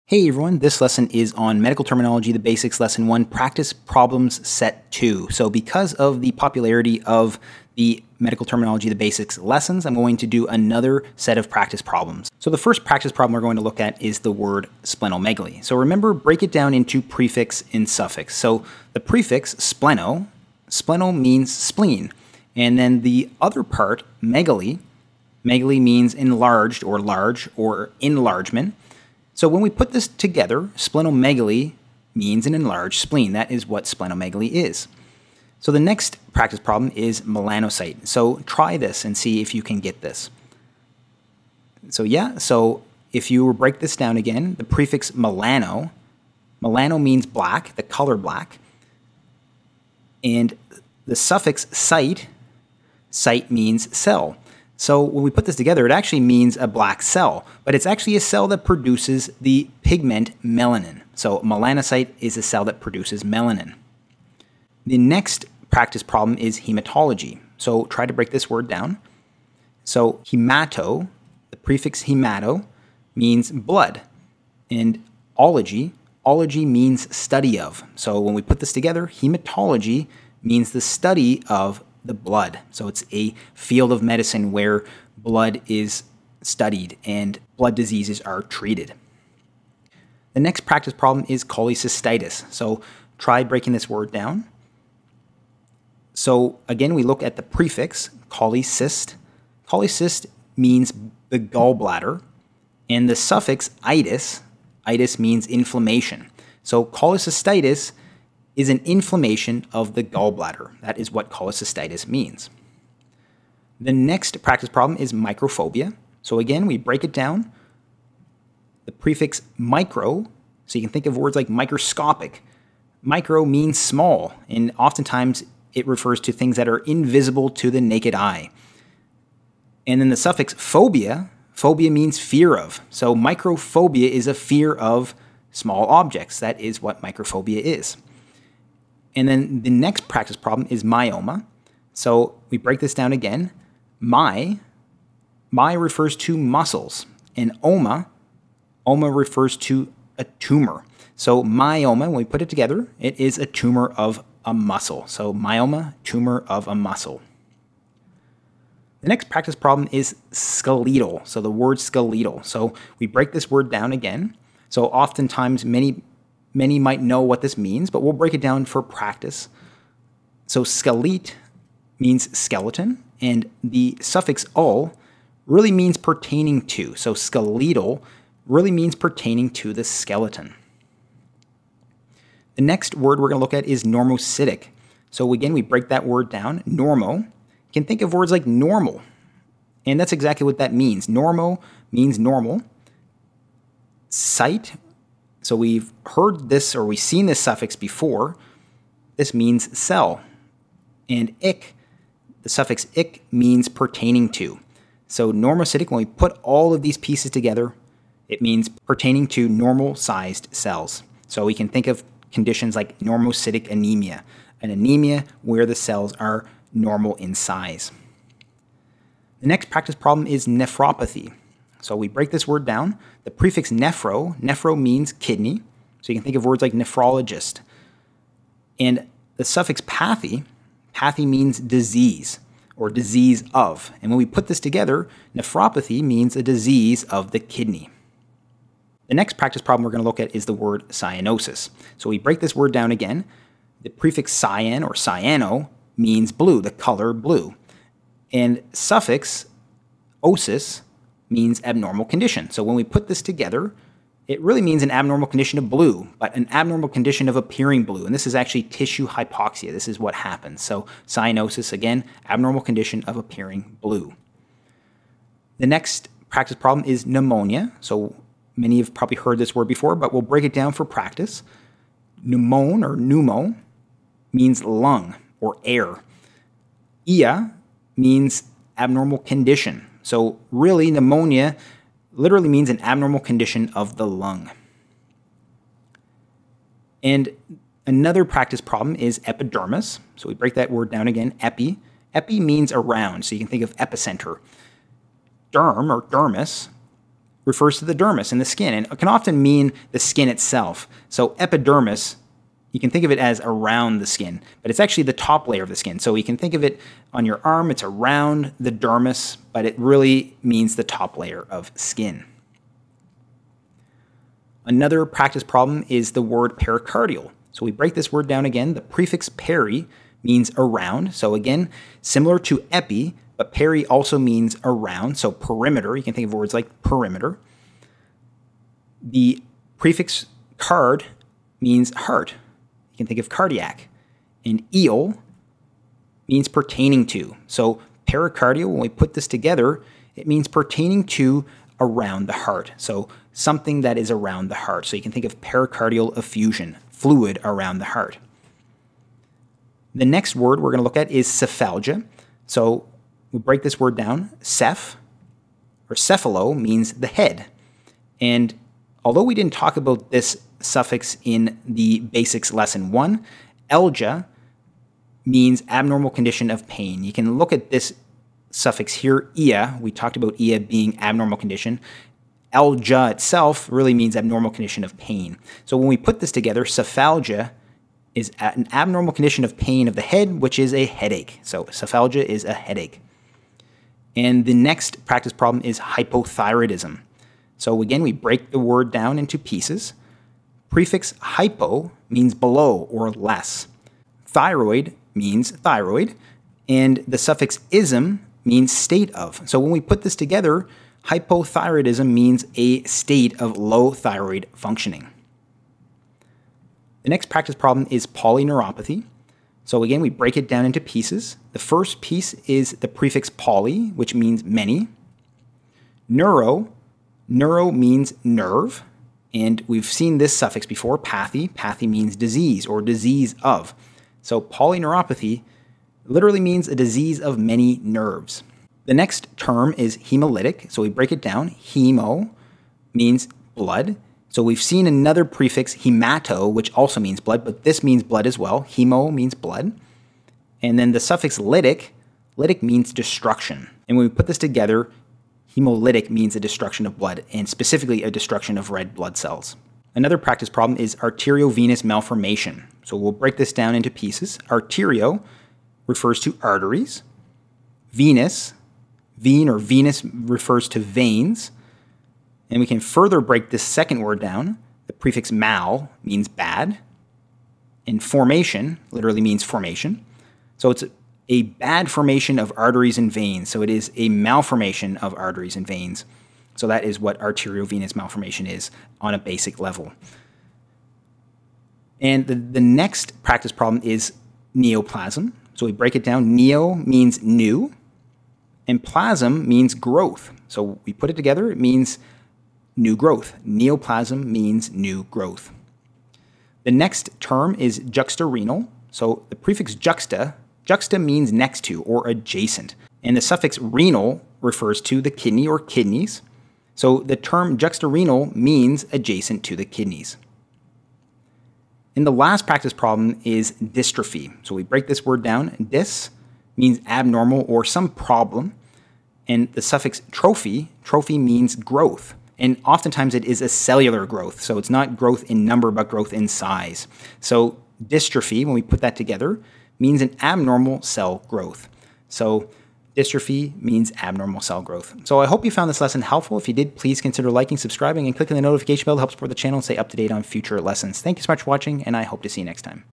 Medical Terminology – The Basics – Lesson 1 | Practice Problems Set 2
Medical Terminology, The Basics, Lesson 1: Practice Problems Set 2. A set of 21 medical terminology practice problems (from Medical Terminology The Basics Series) for EXTRA practice on The Basics (including organs, locations, etc.).

This video is all about breaking down medical terms in a step-by-step way so that you have the future experience to break down other medical terms you see in the future. Please let me know if you want more videos like this one :)

JJ

*FOR BOOKS ON THIS TOPIC*
Amazon Store
Best Way to Remember and Memorize Medical Terms
Medical Terminology For Dummies
Medical Terminology Flash Cards